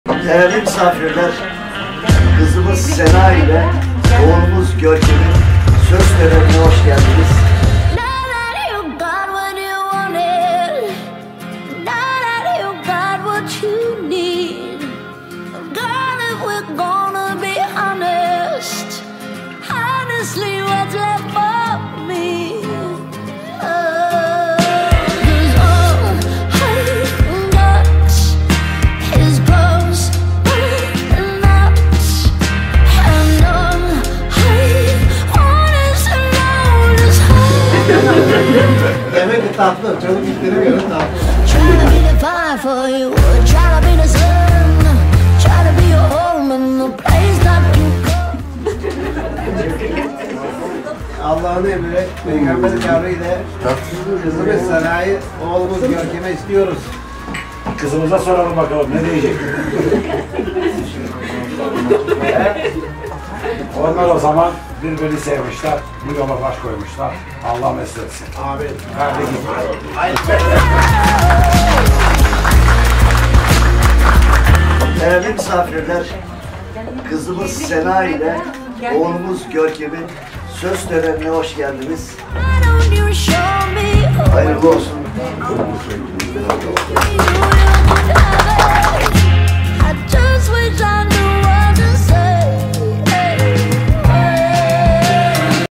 Sena ✨ Görkem
Sevgili Sena ve Görkem ile Velimeşe'de evde çekimini gerçekleştirdiğimiz söz video klibimiz sizlerle.

Konum📍: Tekirdağ/Çerkezköy